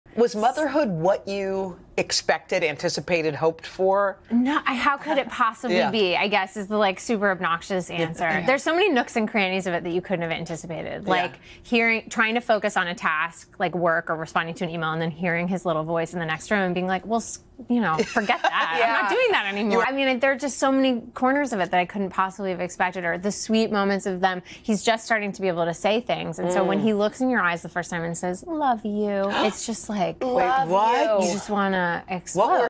Allison Williams on motherhood
Hoda and Jenna caught up with actor Allison Williams about being a mom to her two-year-old son Arlo. She also talked about her latest project, "Erased," a true crime radio series that chronicles the first recorded murder trial in the United States.